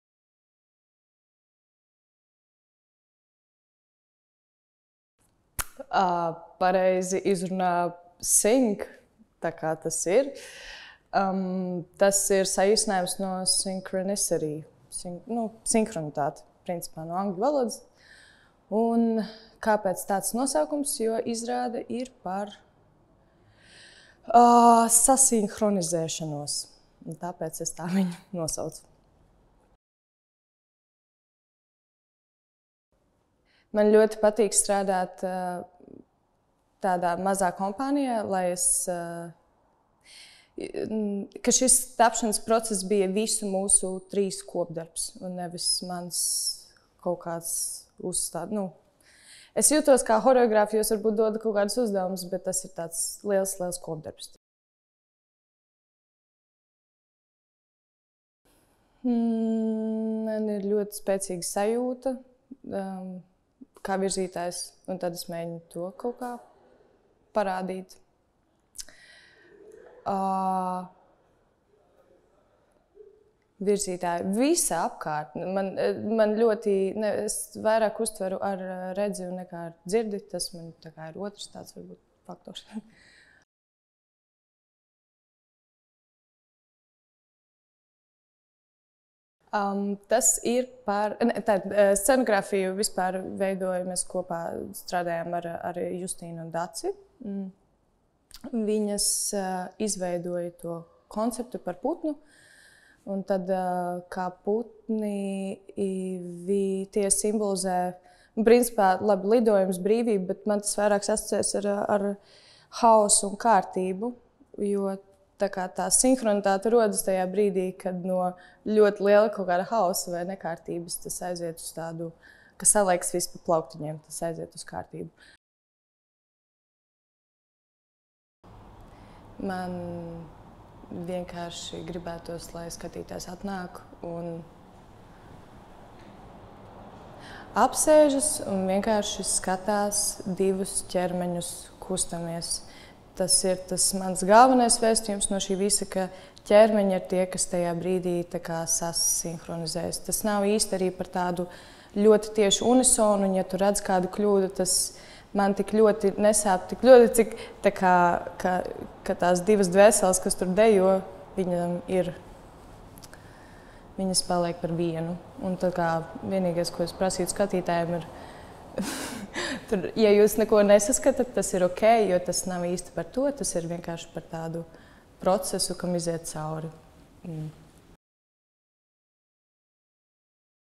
PATRIARHA RUDENS #12 / Līva Apšeniece
PATRIARHA RUDENS #12
Intervija ar izrādes "SYNC" horeogrāfi Līvu Apšenieci

Intervē Krista Skudra
Operators Miķelis Liniņš